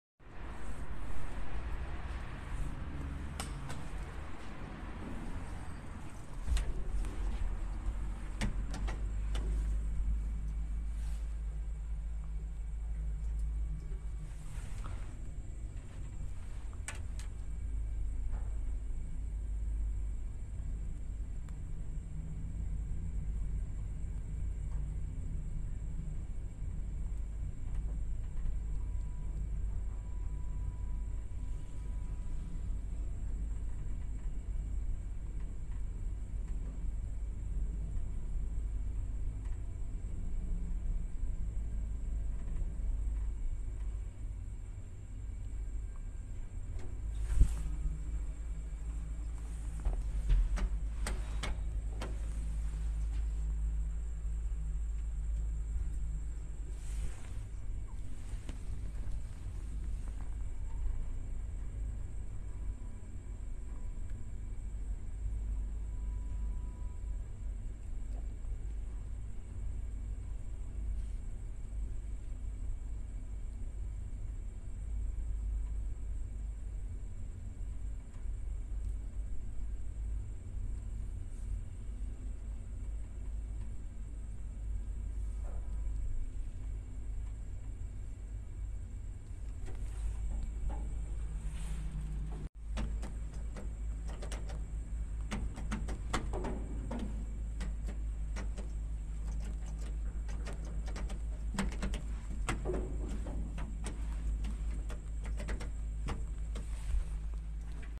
[キャンセル可/簡易更新]布施駅付近某ビルのエレベーター(再撮影)
メーカー:フジテック
機種:ロイヤルスーパーダインインペリアル
用途:乗用
定員:9名
積載:600kg
設置数:1